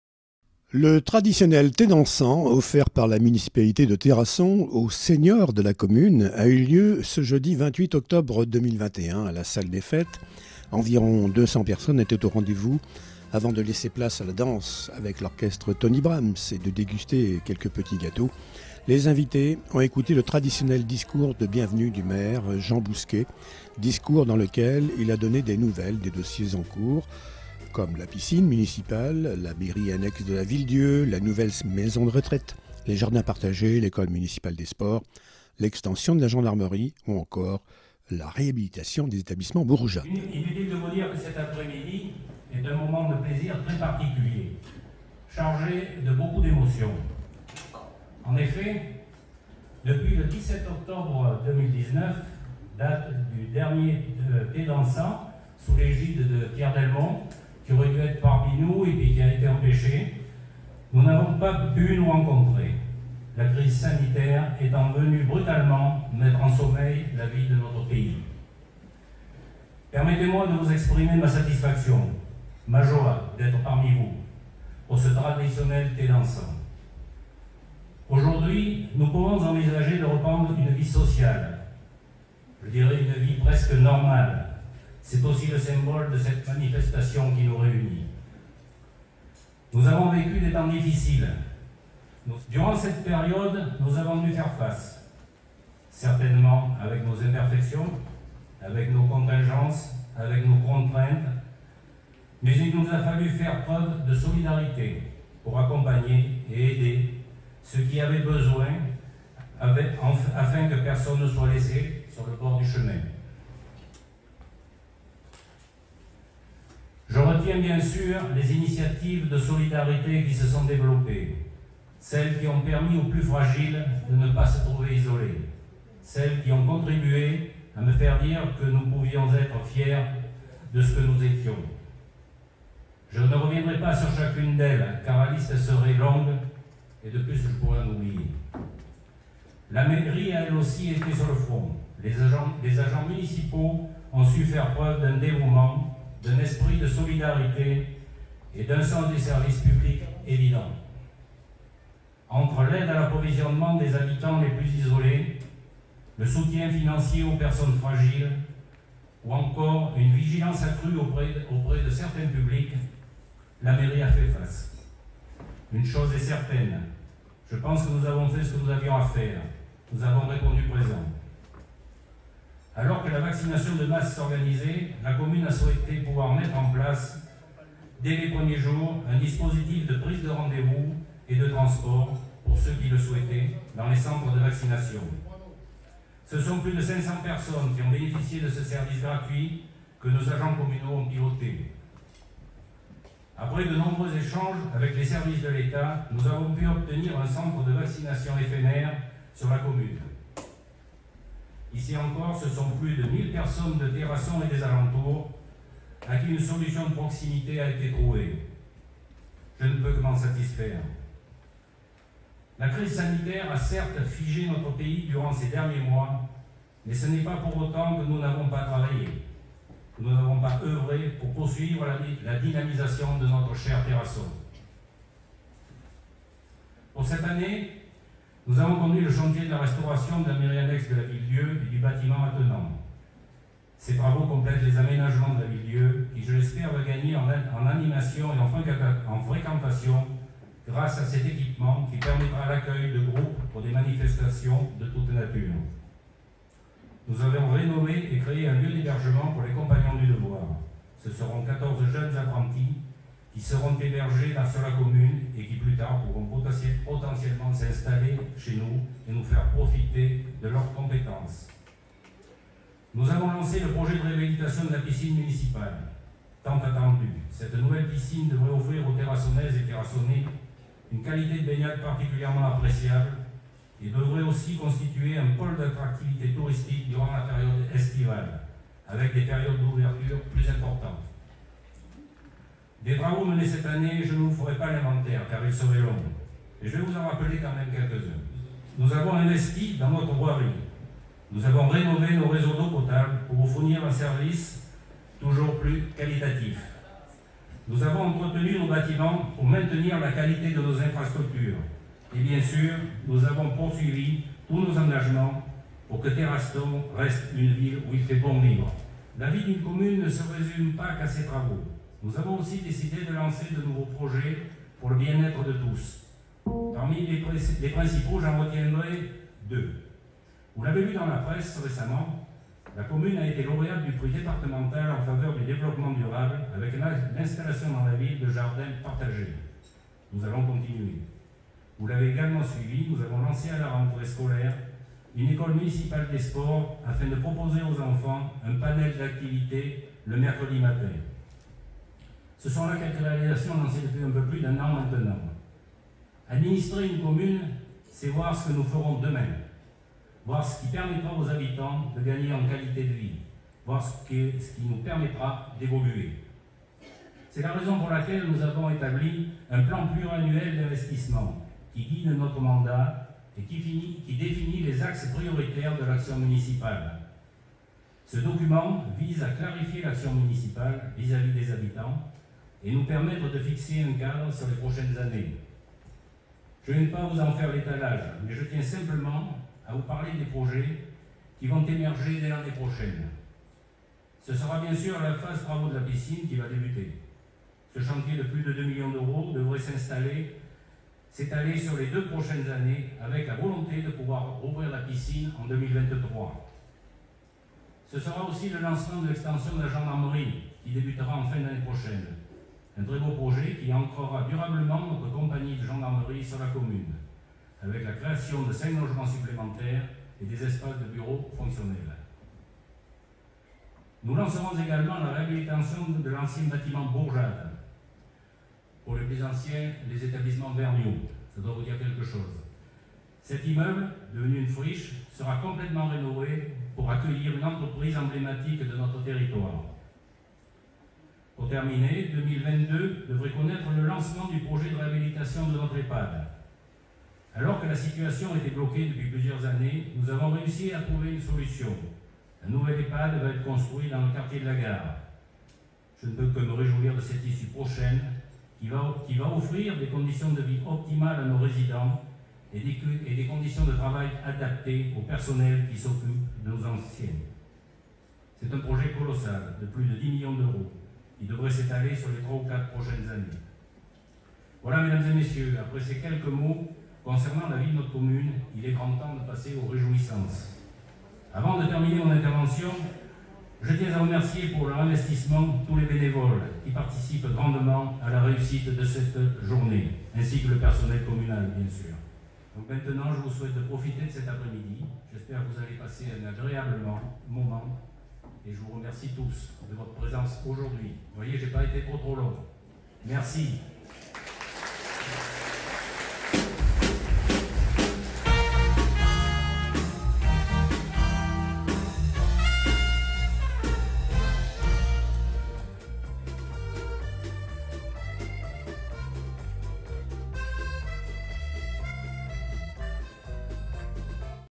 Terrasson, la Ville invite les seniors
Le thé dansant de la municipalité de Terrasson offert aux seniors de la commune ce jeudi après-midi s'est déroulé sous la forme d'un thé dansant animé par l'orchestre Tony Brahm's. Le maire Jean Bousquet n'a pas échappé au traditionnel discours de bienvenue avant de laisser la place aux petits gâteaux et à la danse. Il a donné des nouvelles des dossiers en cours comme la piscine et la nouvelle maison de retraite...